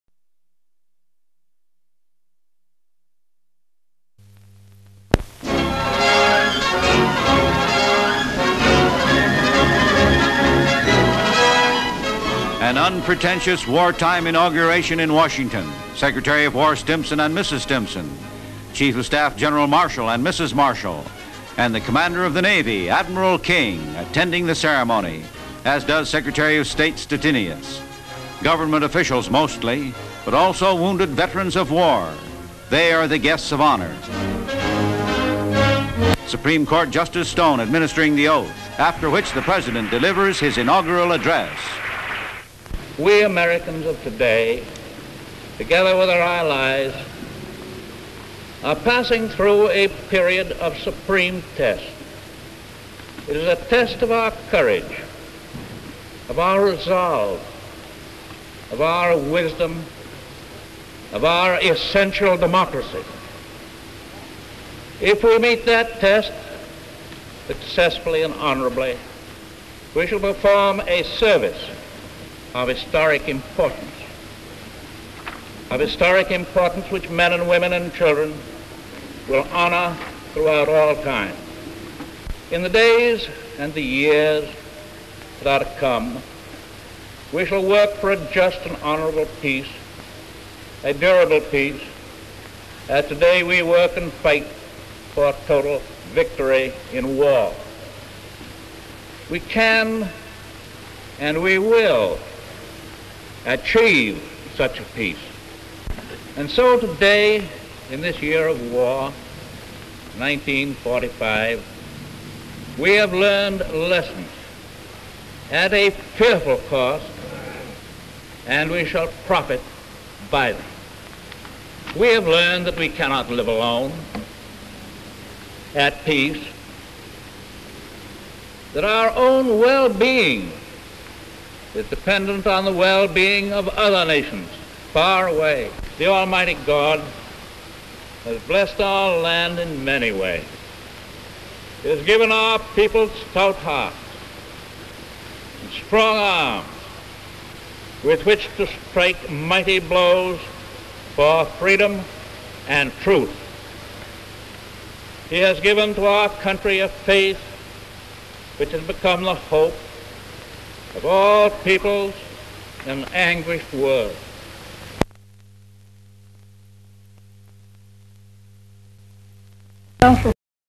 ROOSEVELT INAUGURATION - SOUND
Various shots of guests arriving in grounds of White House, including wounded troops. Roosevelt takes oath (silent) Speech in sound to finish by Roosevelt. The inauguration took place on the balcony outside the White House, Roosevelt son was present.